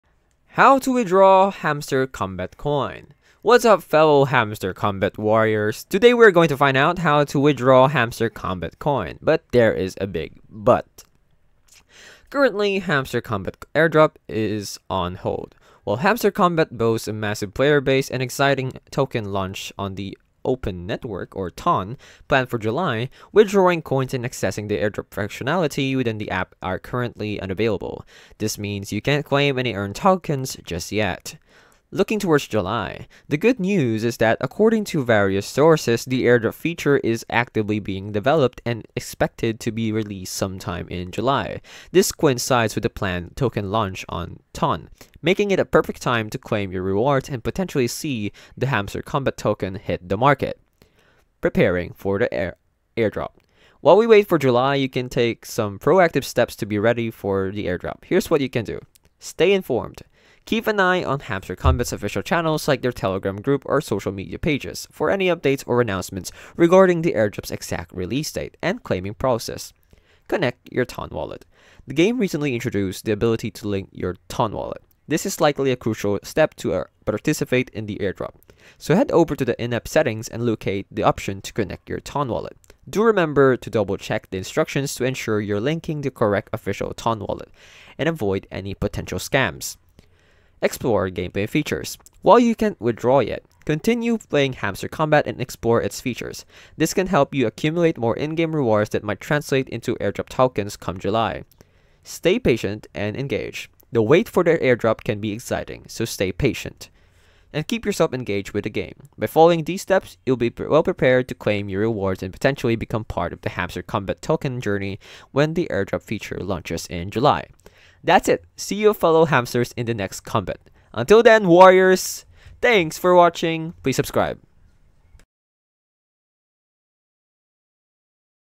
How To Withdraw Hamster Kombat Coin (Best Method)
How To Withdraw Hamster Kombat Coin

In this video I'll show you how to withdraw coins in hamster kombat. The method is very simple and clearly described in the video. Follow all of the steps & withdraw hamster kombat coin

That's How you can withdraw hamster kombat coin